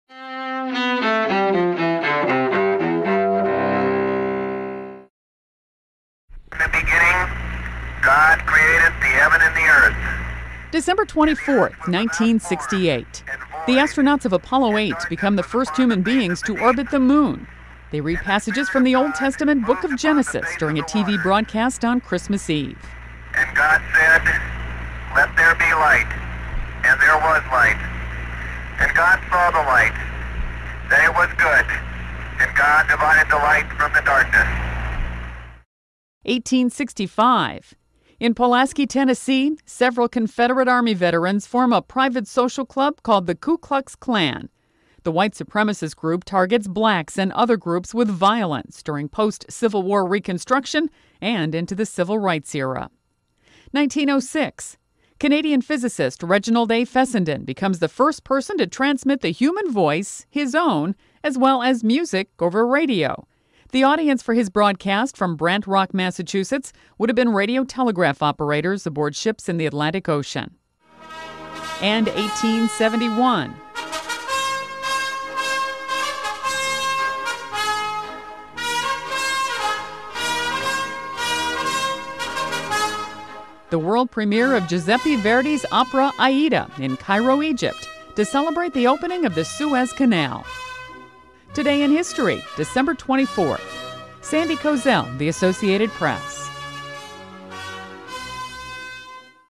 Today in History for December 24th
Apollo Eight astronauts orbit the moon;  Ku Klux Klan  is founded;  Human voice first transmitted via radio; Suez Canal opened. (Dec. 24)